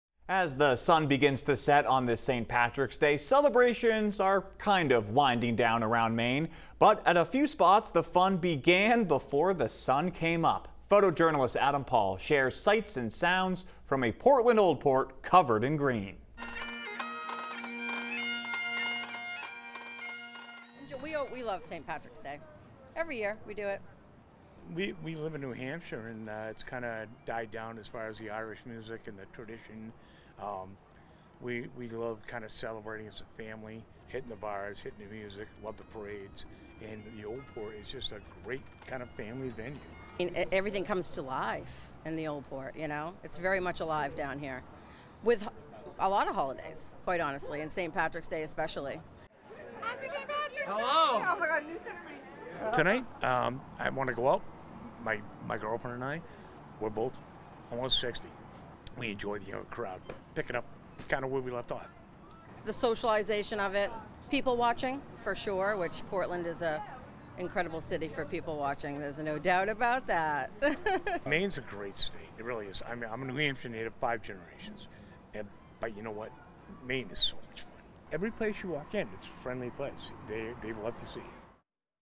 St. Patrick's Day festivities begin before sunrise in Portland
Portland's Old Port was covered in green for the Irish holiday.